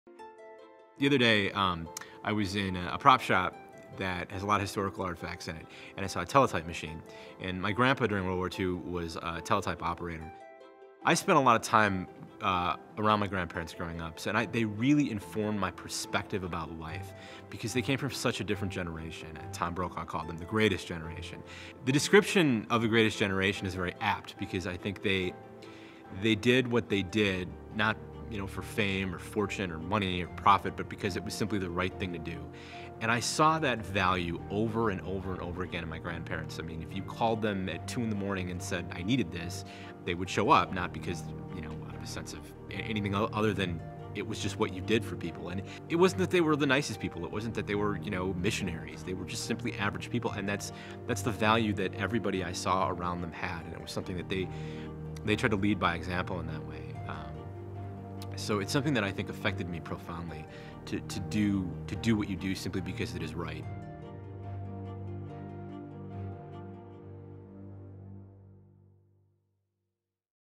What Matters Most: Doing Right
Volunteer TJ discusses what matters most to him.